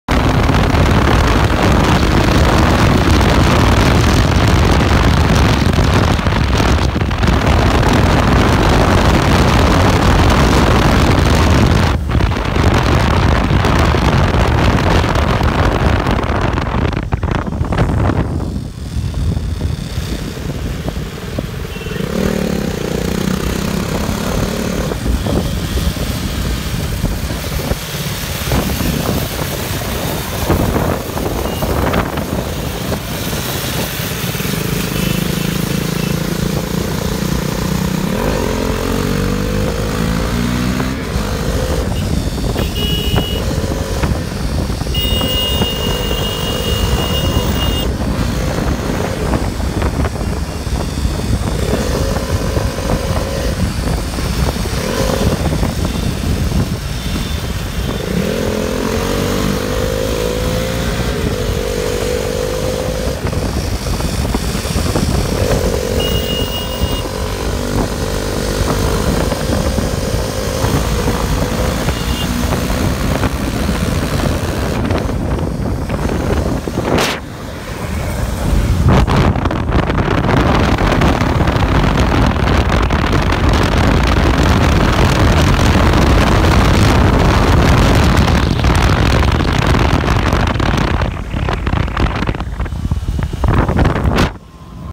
Two KTM 390 Duke racing on streets
In the busy streets of India, two KTM 390 Duke motorcycles were racing, incredible footage.